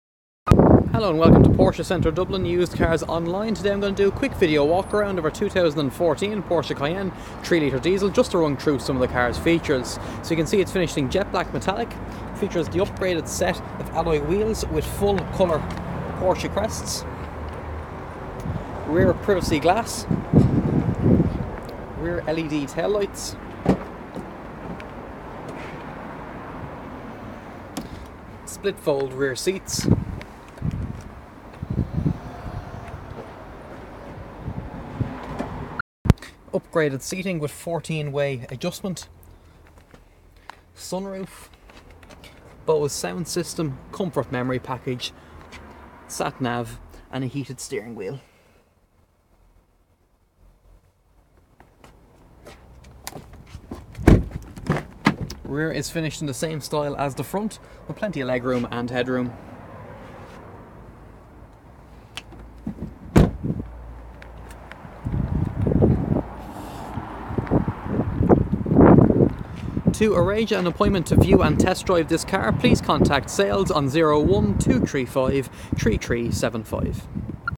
141D4399 - 2014 Porsche Cayenne 3.0 Diesel 71,995
Here is a video of our 141D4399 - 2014 Porsche Cayenne 3.0 Diesel 71,995. Please contact Sales to arrange a test drive today. Porsche Centre Dublin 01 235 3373 infoporschedublin.ie